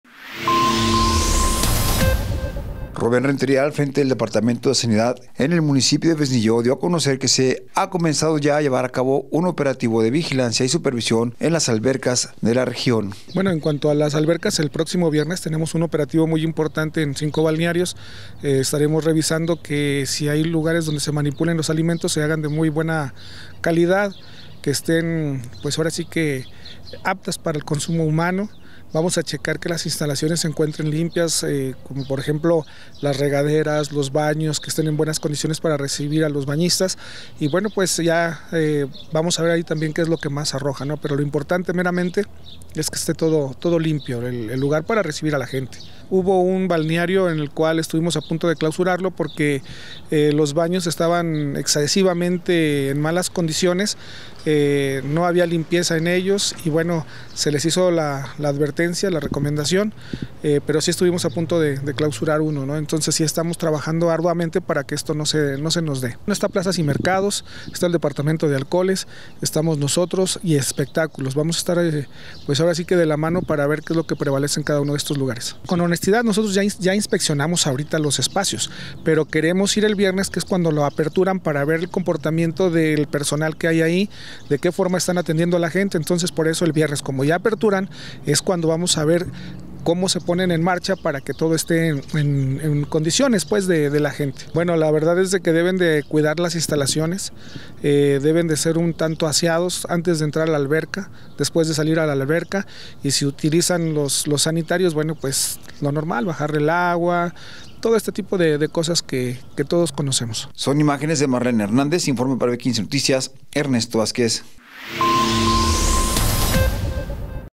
Realizarán Operativos de Sanidad en Balnearios de Fresnillo para Garantizar Condiciones Óptimas
Fresnillo, Zacatecas.- Rubén Rentería Elizalde, titular del Departamento de Sanidad en Fresnillo, anunció el inicio de un operativo de vigilancia en las albercas de la región. Este operativo, que se llevará a cabo el próximo viernes en cinco balnearios, tiene como objetivo garantizar la calidad de los alimentos ofrecidos, así como la limpieza y condiciones sanitarias de las instalaciones.

"Estaremos revisando que si hay lugares donde se manipula en los alimentos se hagan de muy buena calidad que estén pues ahora sí que aptas para el consumo humano. Vamos a checar que las instalaciones se encuentren limpias, como por ejemplo las regaderas, los baños, que estén en buenas condiciones para recibir a los bañistas"

Rentería enfatizó la importancia de que las albercas cumplan con estándares de higiene adecuados para el consumo humano. Se revisarán detalladamente áreas como regaderas, baños y zonas de manipulación de alimentos. Además, se hizo hincapié en la necesidad de mantener las instalaciones en óptimas condiciones para recibir a los bañistas, destacando la importancia de la limpieza y el cuidado de los espacios comunes.